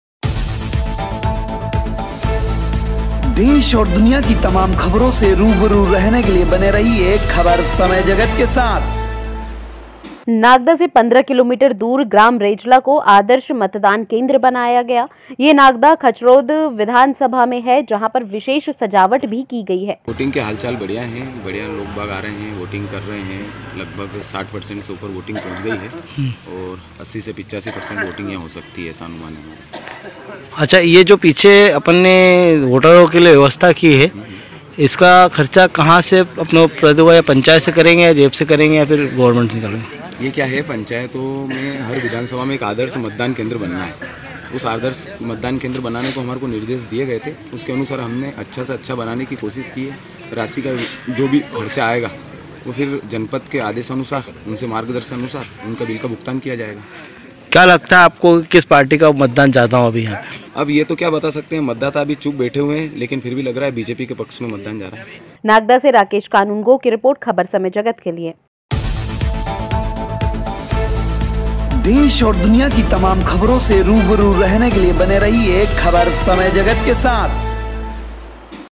नागदा - नागदा से 15 किमी दूर ग्राम रेजला को आदर्श मतदान केंद्र बनाया गया
राकेश क़ानूननगो की रिपोर्ट